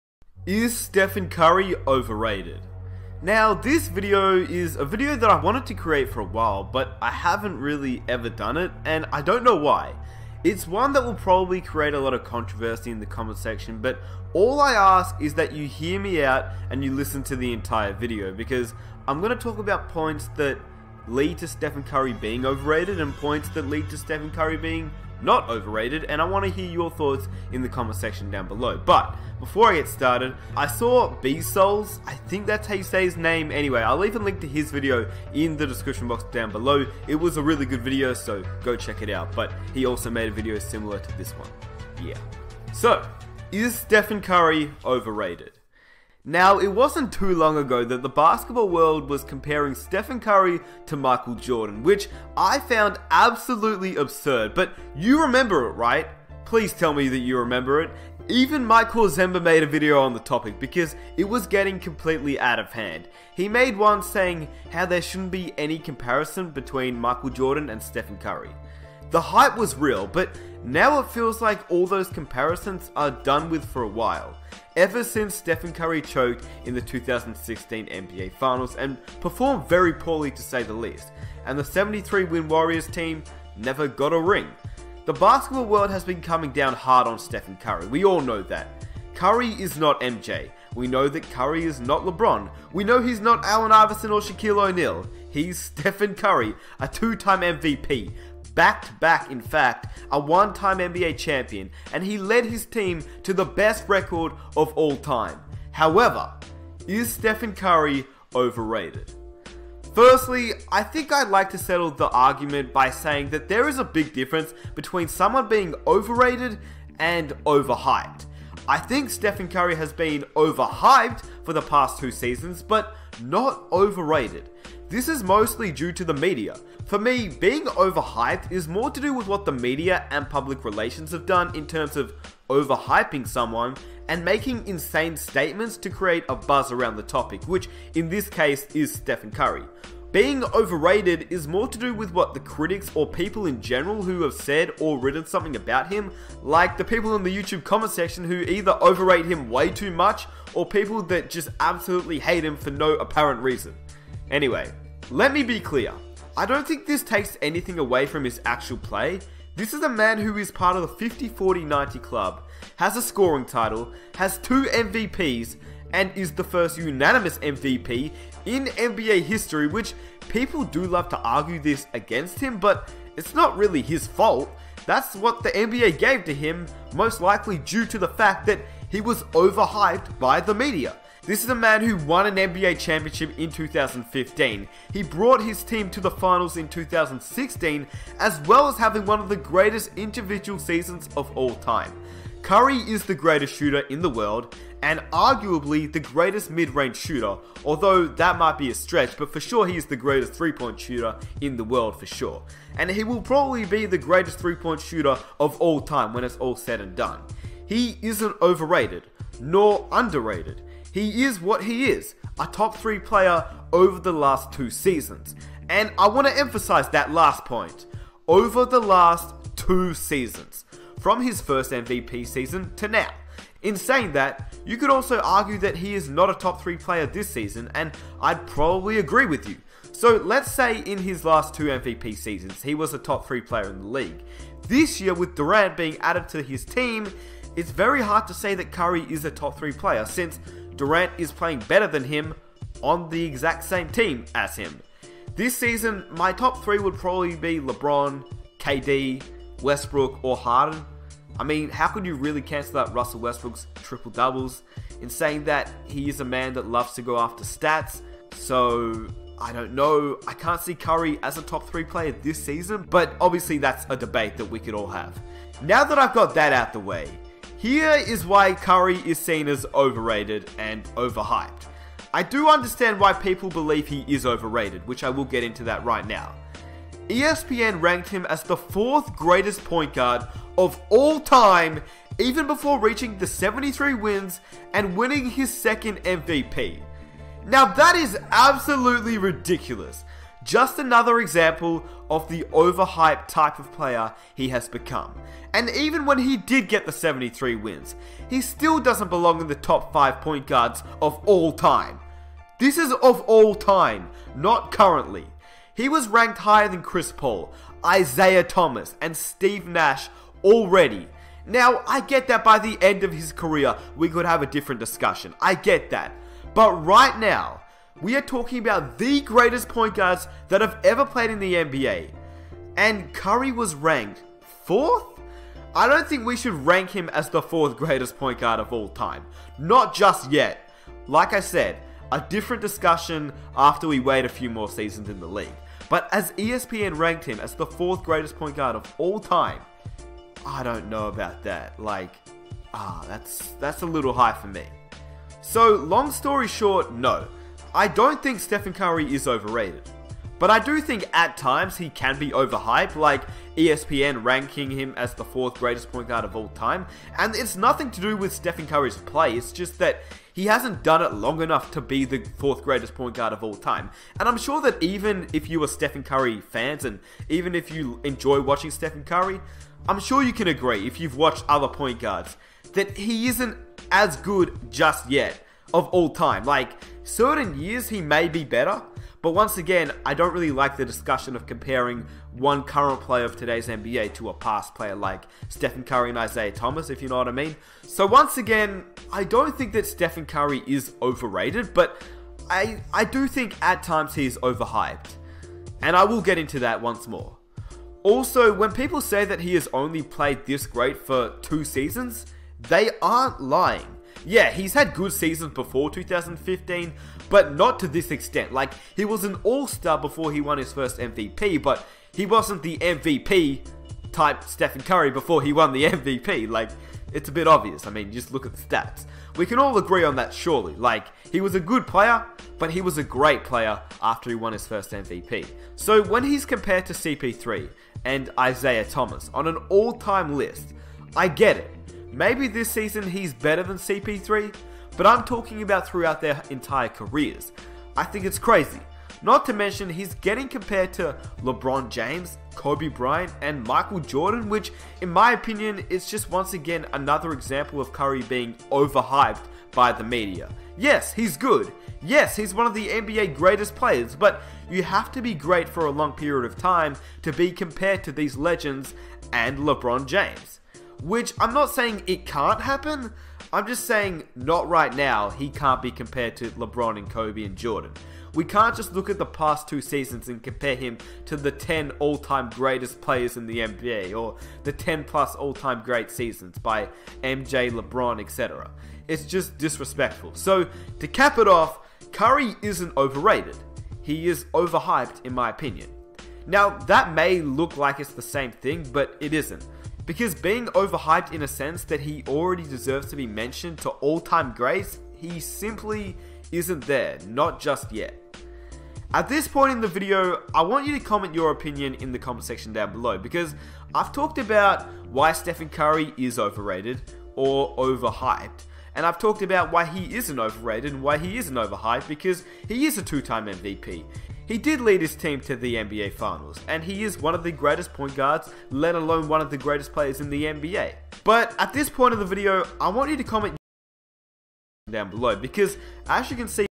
Is Steph Curry overrated?Should he be compared to Michael Jordan?
Is Steph Curry overrated?

Youtube Video Credit by:Nick Smith NBA